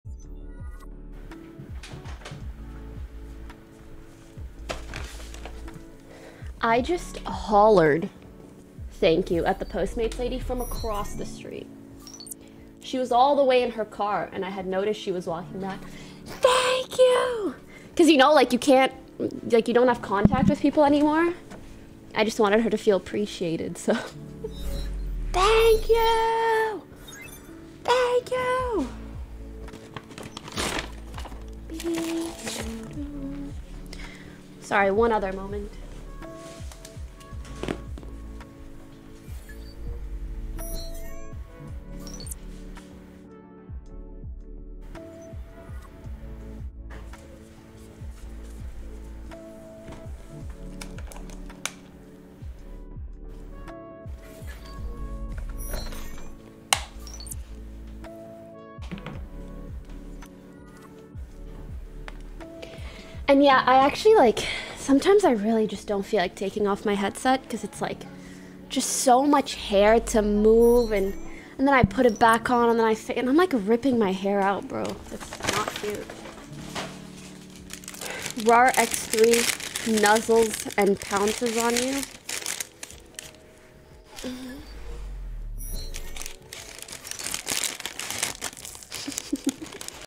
Pokimane Say Thank You to Postmates
Recently, Pokimane ordered food form postamate so this is video for postmate. in this video i am gonna show you what she said and what she thinks about drawing.

►Credits to Pokimane
►Pokimane :  @Pokimane